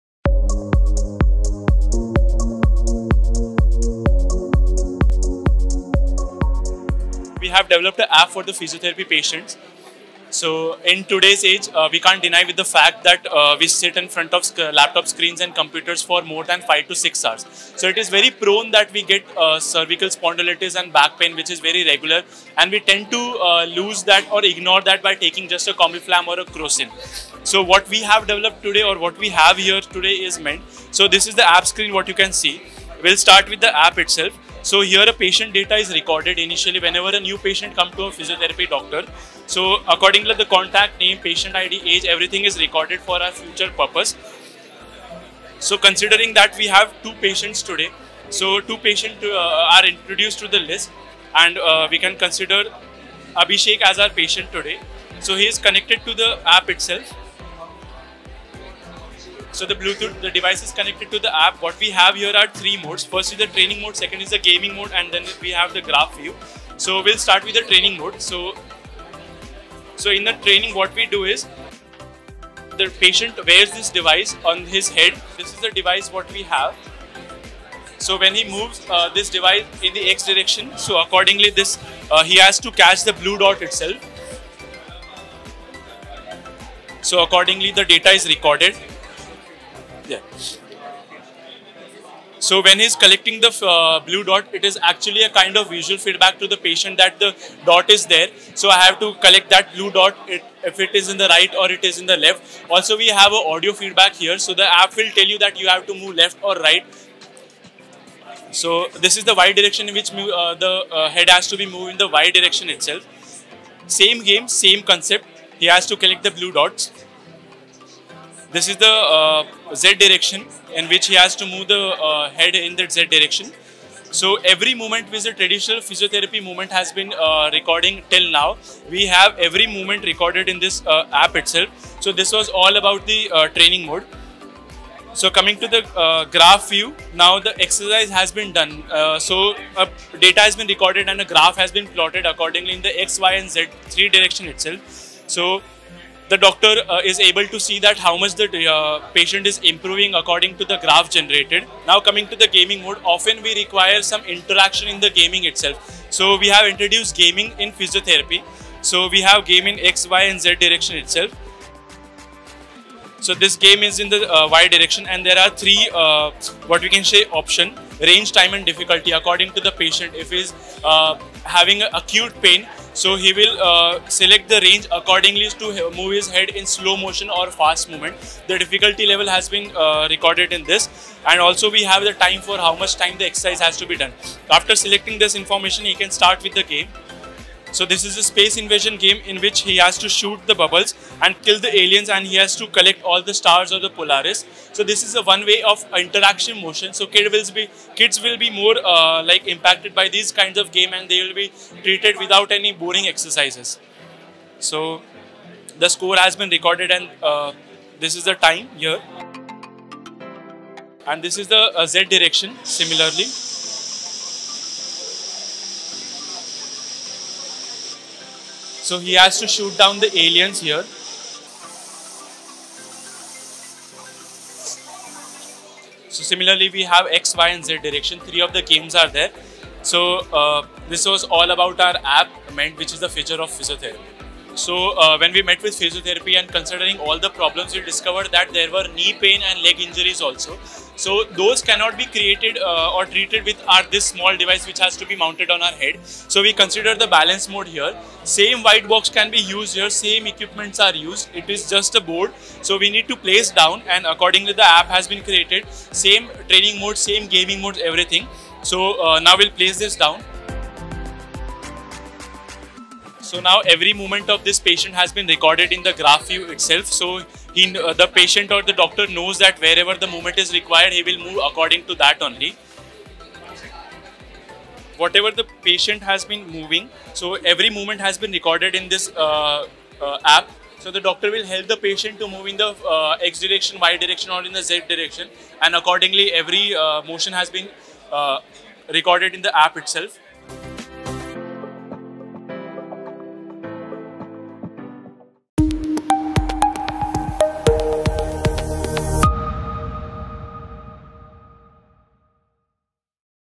eYIC 2019-20: Physiotherapy Portable Cervical Joint Assessment Device with Feedback System
Team from K J  Somaiya College of Engineering, Mumbai selected for the National Finals of eYIC 2019-20 .